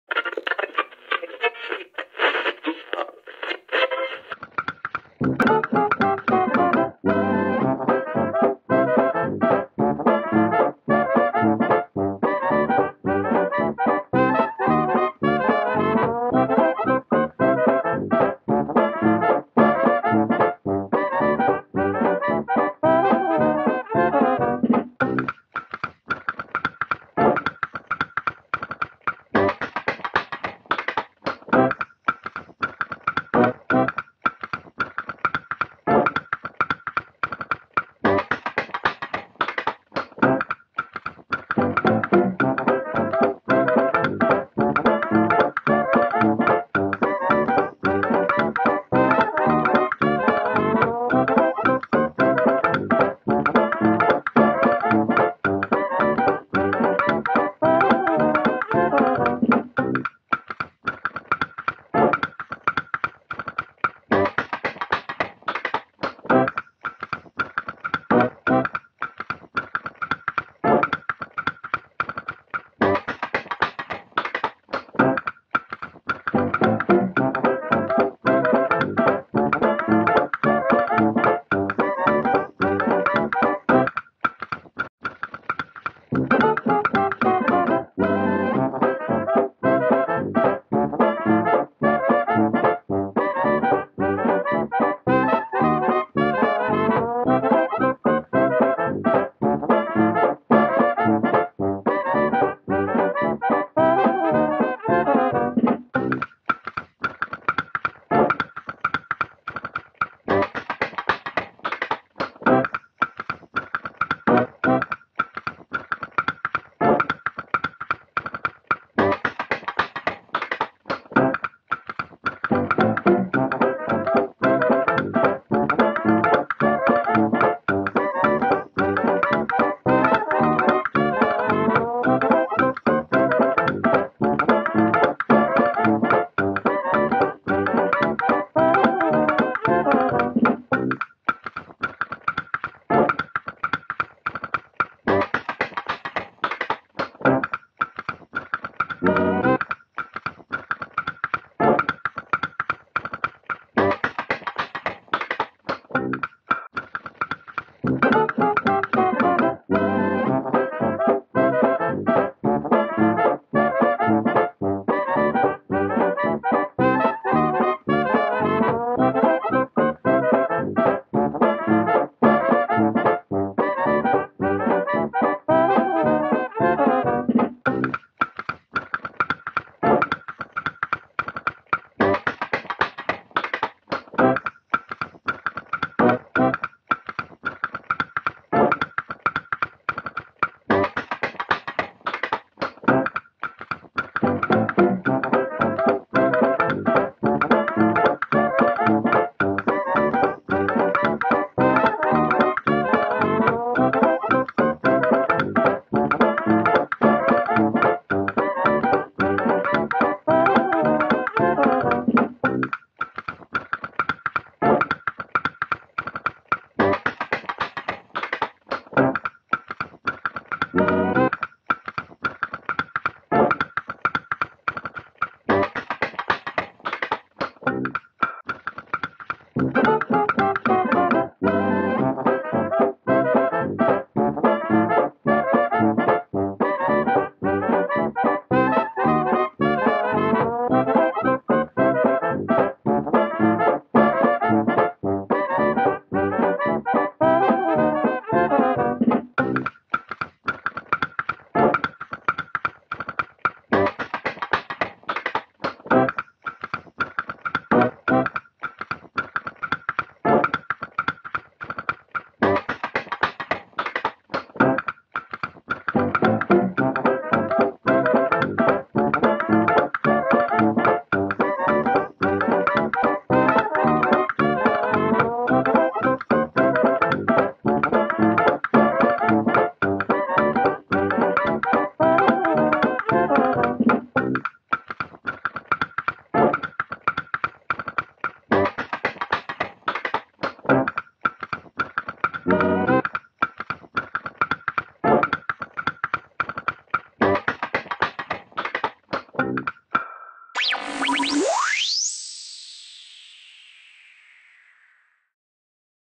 5 Minute Timer with Music: Animated Record Player Turntable
This 5-minute timer counts down from 5 minutes to zero. Every minute, the colors shift as the record spins and music plays. The countdown begins with some light tuning noise and ends with an indicator that time is up.

This timer works as an indicator for the duration of a break, a countdown to an online class or meeting, as the background to 5 minutes of chores, or for a dance break to your day.